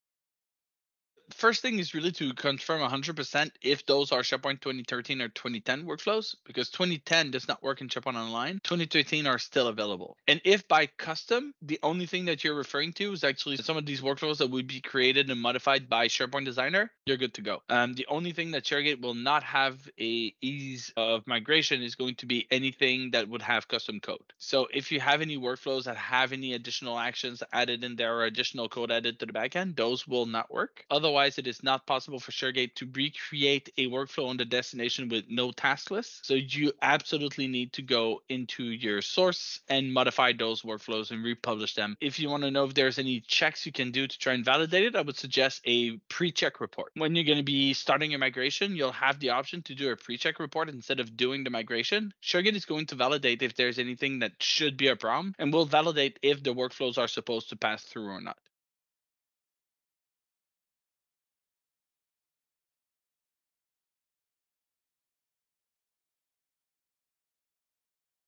Copy Workflow
Copying workflows makes them available at the destination site. The only supported workflows are the built-in SharePoint workflows, those created with SharePoint Designer, and Nintex Workflows.